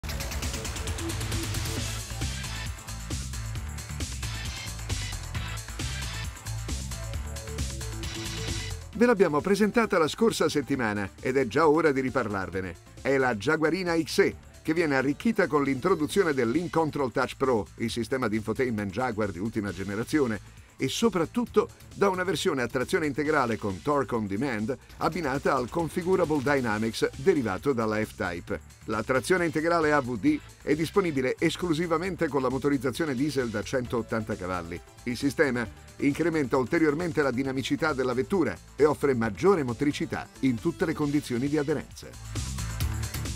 Jaguar XE AWD | News di Autolink
Ve l’abbiamo presentata la scorsa settimana ed è già ora di riparlarvene. È la Jaguarina XE che viene arricchita con l’introduzione dell’InControl Touch Pro, il sistema d’infotainment Jaguar di ultima generazione e soprattutto da una versione a trazione integrale con torque on-demand abbinata al Configurable Dynamics derivato dalla F-Type. La trazione integrale AWD è disponibile esclusivamente con la motorizzazione diesel da 180 CV. Il sistema incrementa ulteriormente la dinamicità della vettura e offre una maggiore motricità in tutte le condizioni di aderenza.